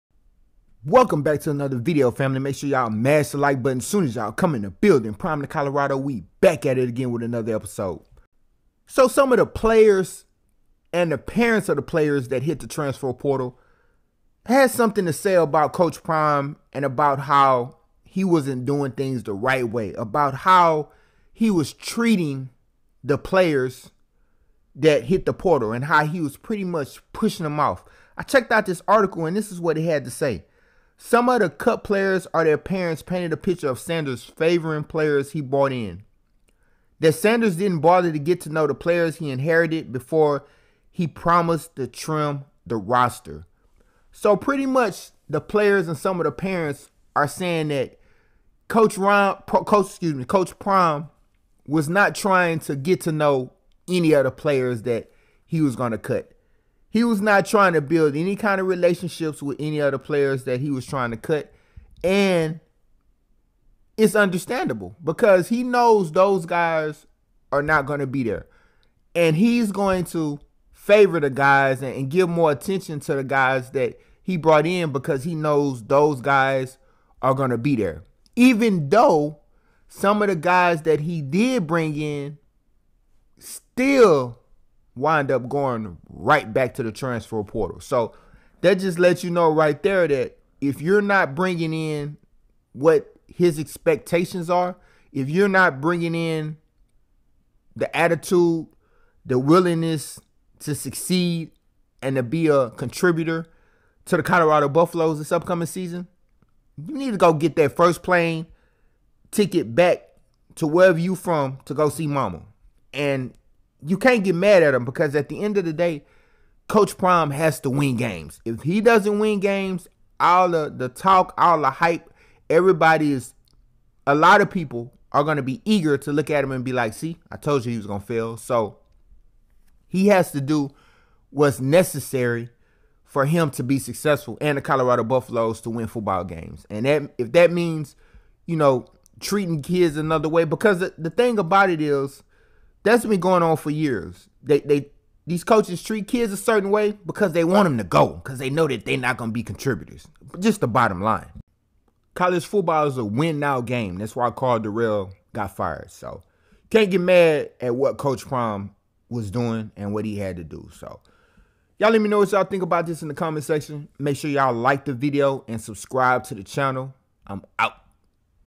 Coach Prime Accused Of Doing This By Transfer Players And Parents ‼️
Transfer players and there parents are saying coach Prime did these things to run them out of the program

They are saying he ignored them and made them feel uncomfortable